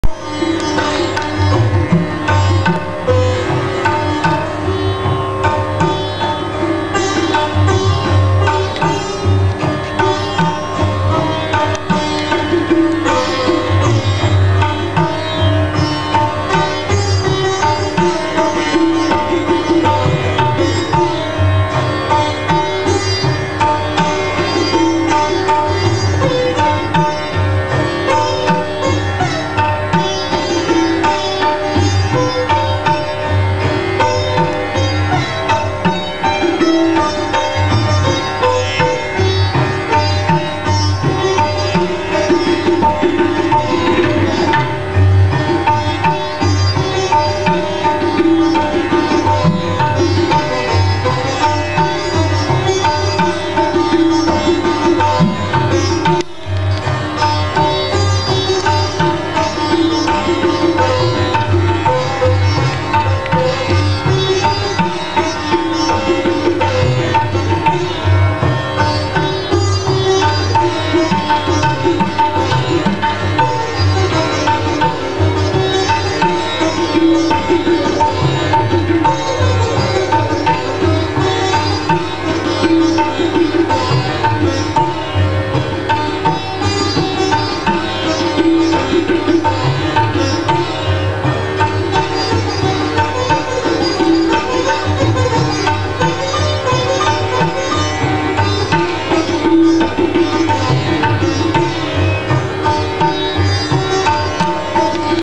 Dr. Aruna Mathur Sitar Reciatal
Sitar Recital for the High Court Judges.